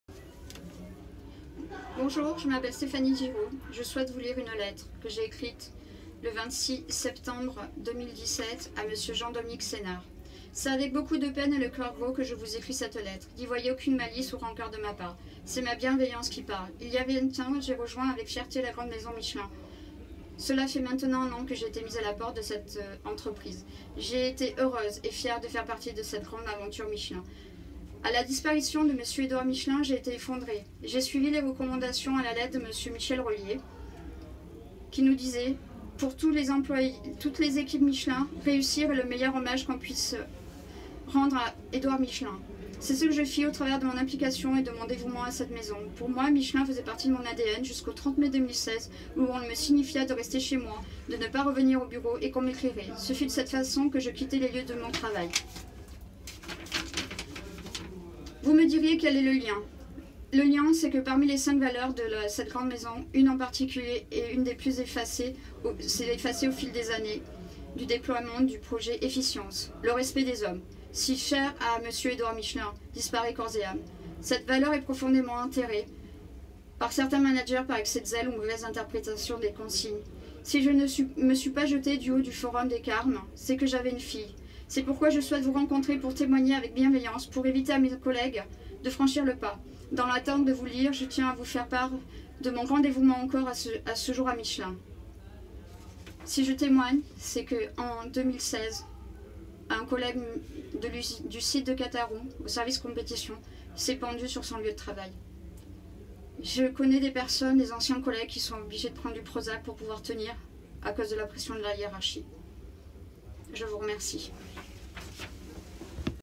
Vidéograph' - Lettre au président du groupe Michelin
A l'occasion de la Convention de la France Insoumise les 25 et 26 novembre, les participants sont invités à intervenir en enregistrant leur contribution dans le Vidéograph'.